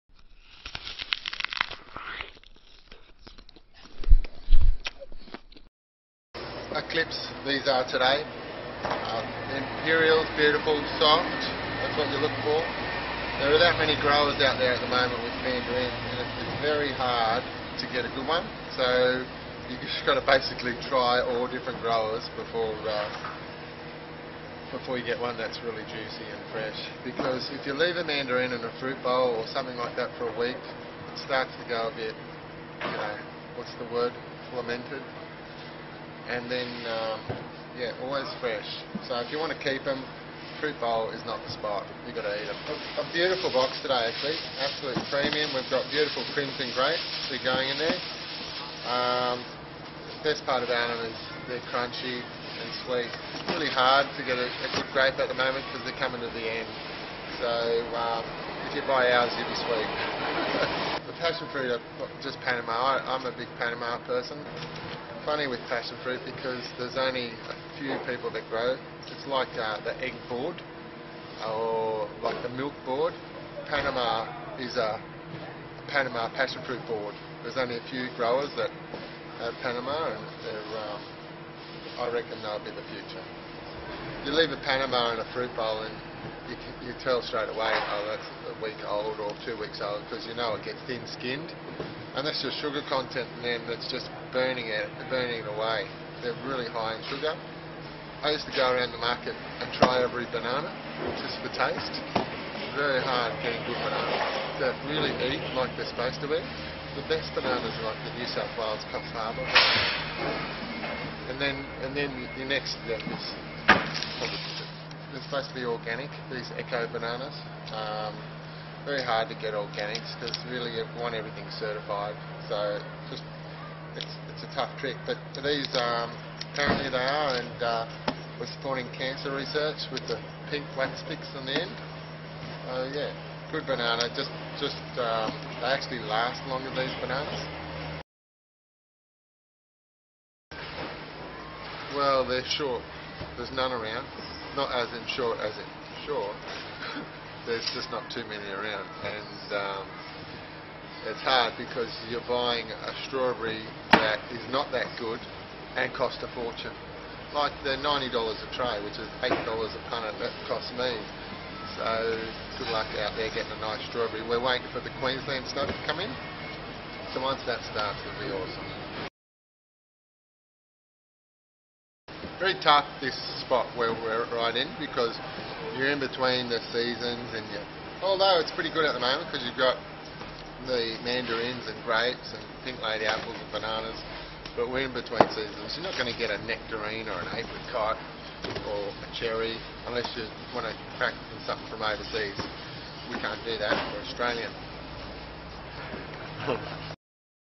FruitMe Fruitcast Episode 4
On this week's Fruitcast, FruitMe talks about Imperial mandarins, Panama passionfruit, organic bananas and Crimson Seedless grapes. Craig also invents a new word - "flemented" - and explains why we aren't seeing many strawberries around at the moment.